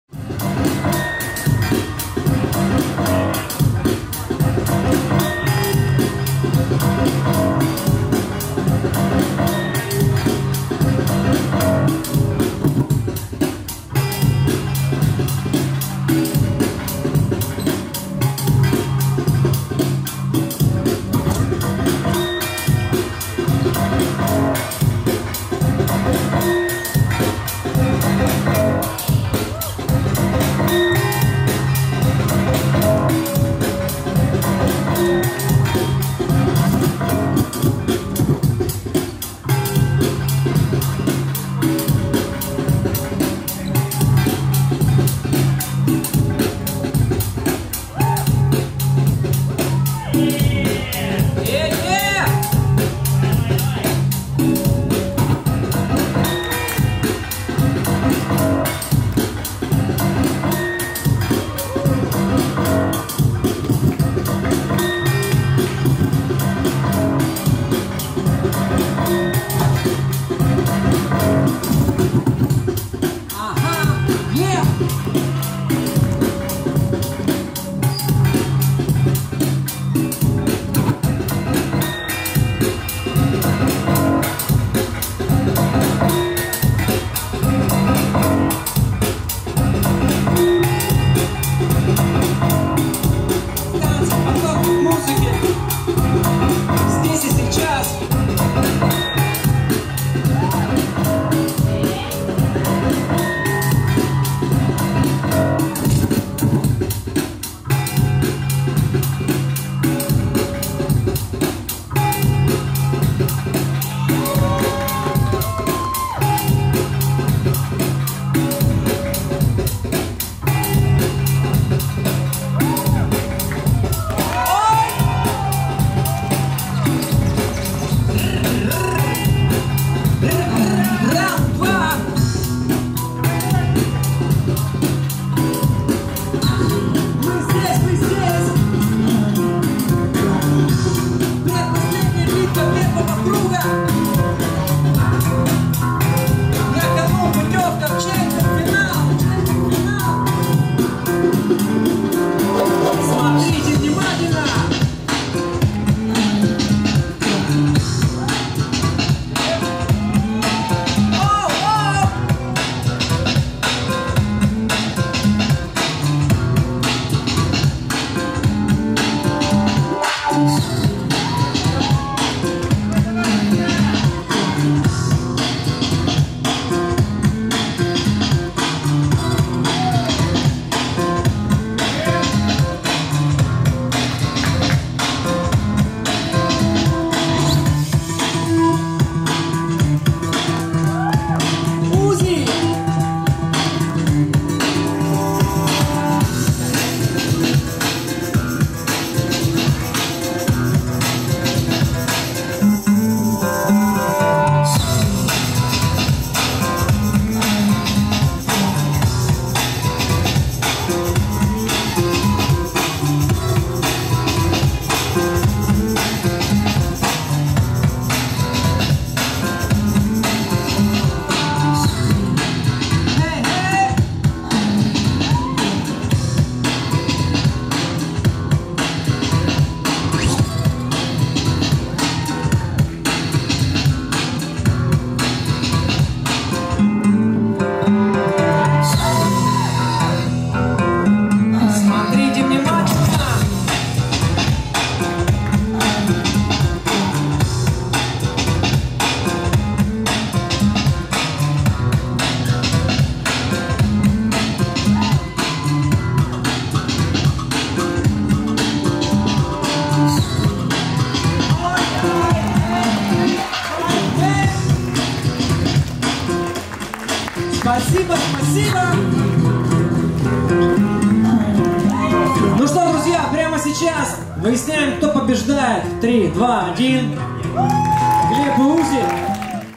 BALANCE VS GLEB & UZI ROCK | 1/8 FINAL
BREAKING BATTLE 2 VS 2 | YALTA SUMMER JAM X EXTREME КРЫМ 2020